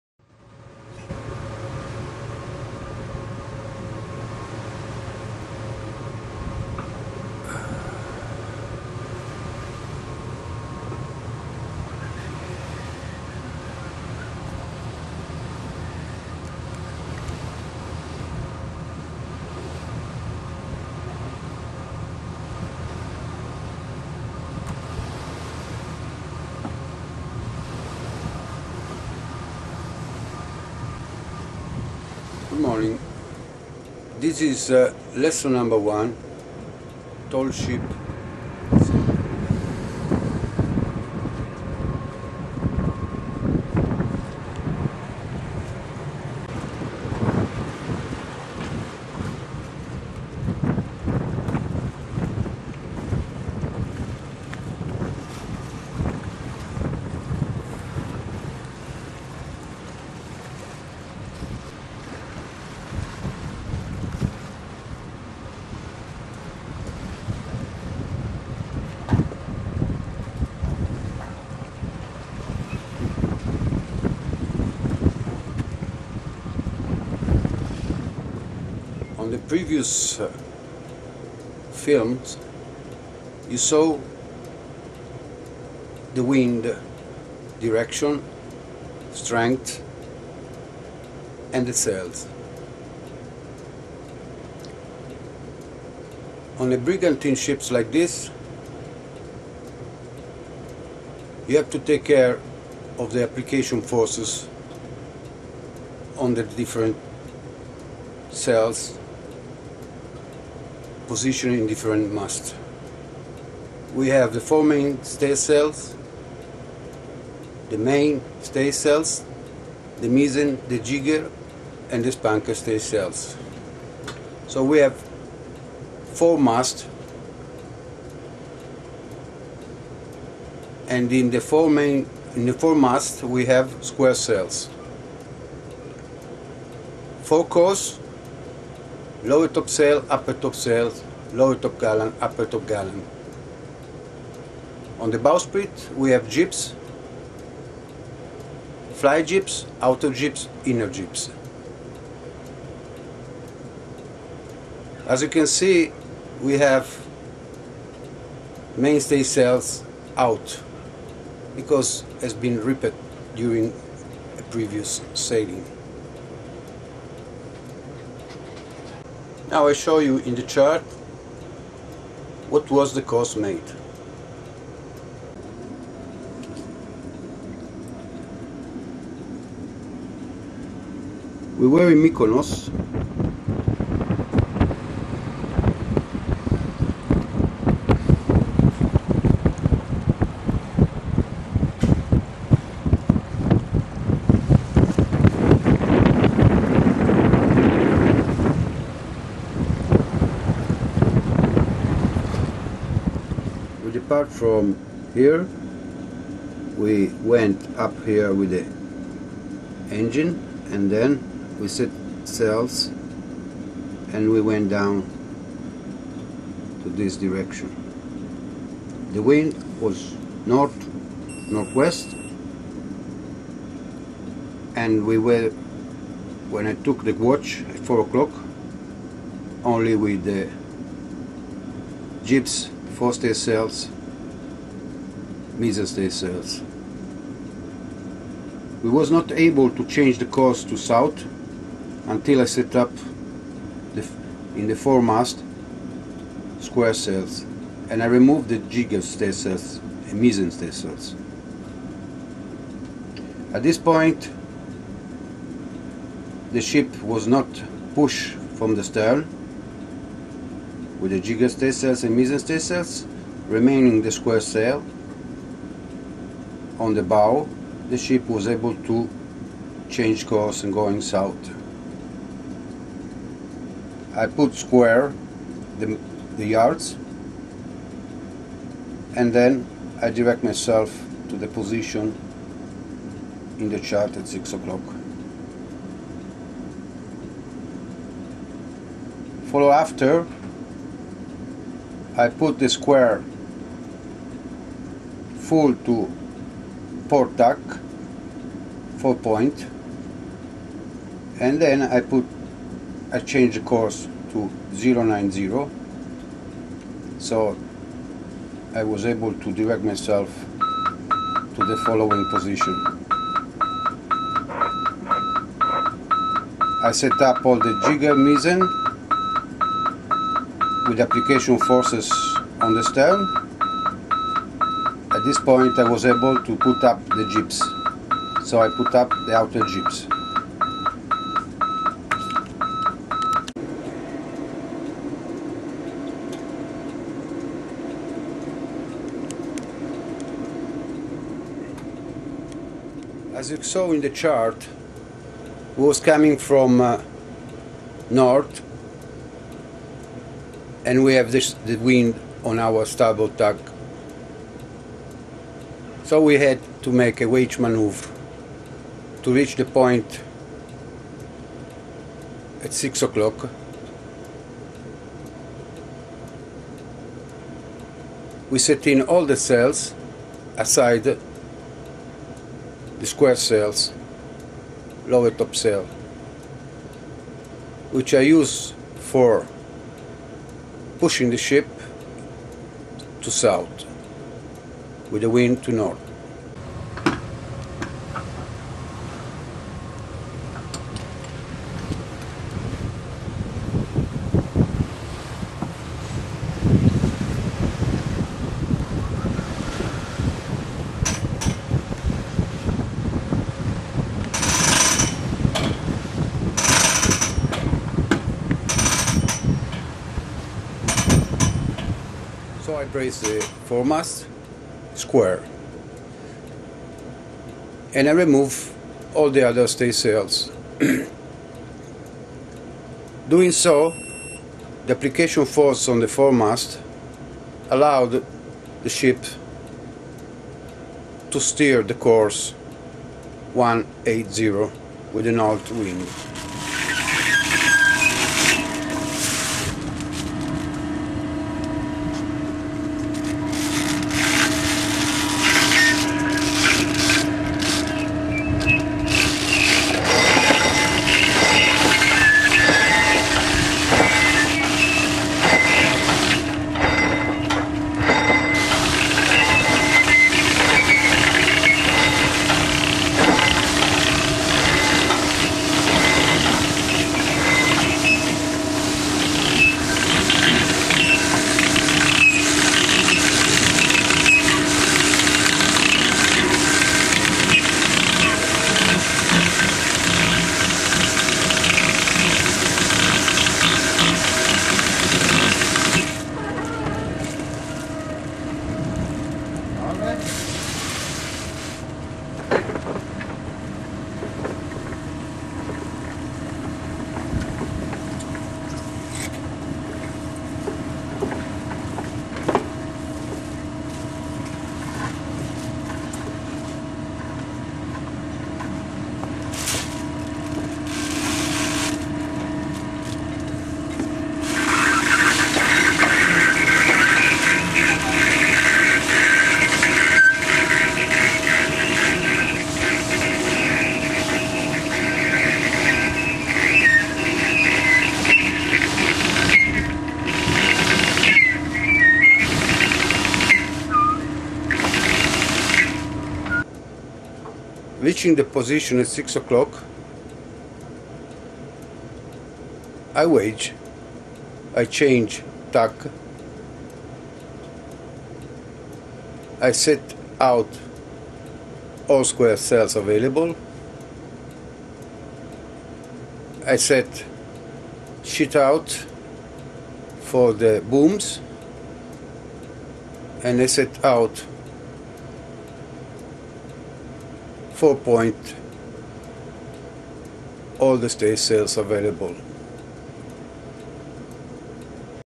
Sailing around Mykonos
Sailing out Mykonos Changing tag, Bracing the fore mast